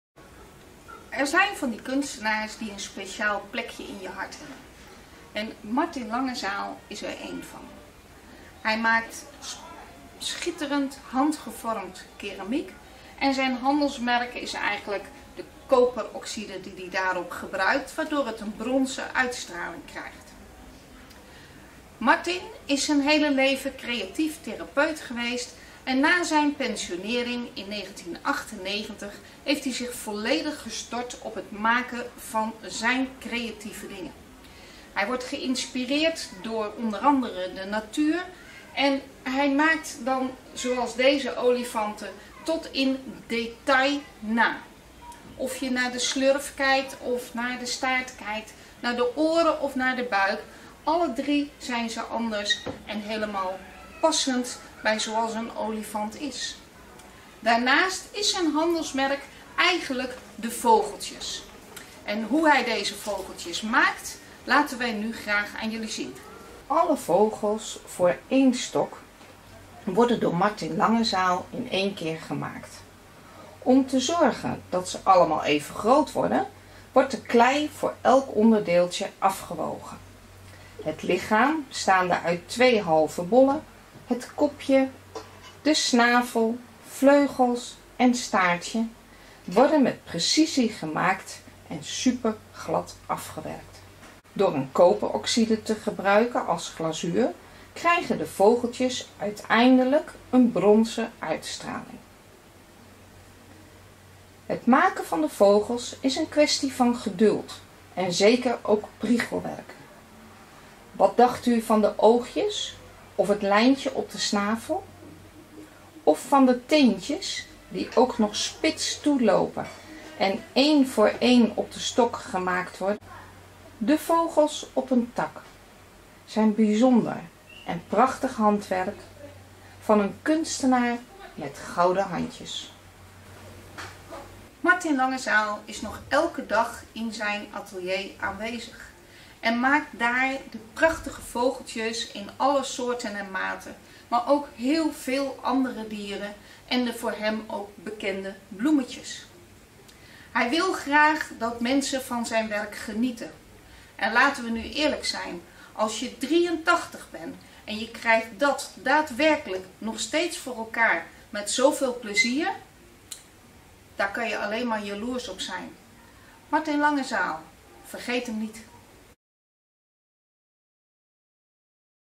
Martin Langezaal - Keramiek
Martin Langezaal maakt handgevormd keramiek. Hij vindt zijn inspiratie in de natuur en maakt meest figuratieve objecten.

Vogels en bloemen, maar ook prachtige olifanten of nijlpaarden behoren tot zijn oeuvre.

Martin is, met zijn 83 jaar,  nog elke dag in zijn atelier te vinden. We hopen dat hij dat nog heel lang kan doen, zodat we van zijn werk kunnen blijven genieten.

Meer informatie over Martin Langezaal en zijn werk vindt u op